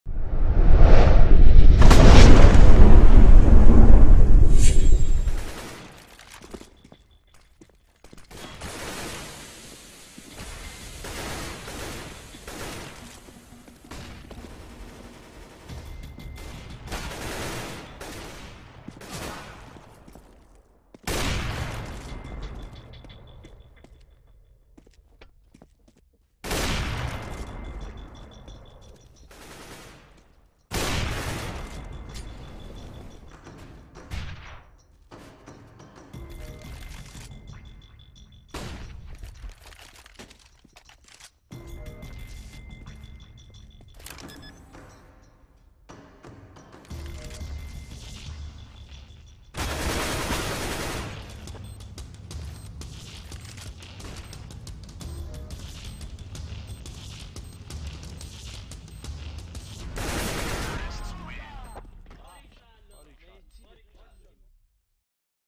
هایلایت مسابقه جام خلیج فارس - RApTOR vs EG - PGC LAN Finals 2014
این بازی توی تهران به صورت لن انجام شده بود که در نهایت تیم ما تونست قهرمان بشه
Map : De_Cache Highlight [Farsi Language]